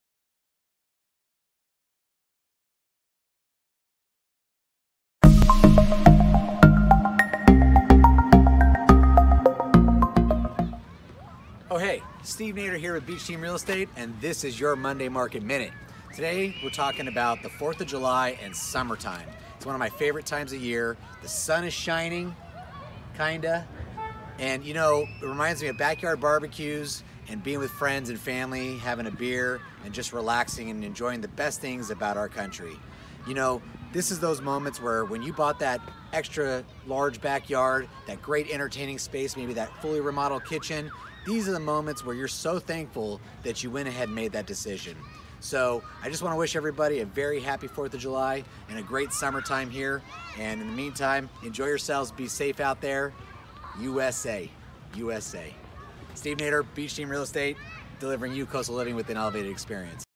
Tip #10 | Happy 4th of July! | Long Beach Real Estate | MMM - Steve Nader
Use this time of year to be thankful you made that decision to purchase those homes with that extra large backyard perfect for hosting your family and friends. Kick off your holiday by barbecuing and relaxing in those beautiful entertaining spaces! Beach Team wishes you a happy and safe holiday! 🇺🇸 Mybeachteam 🌴☀️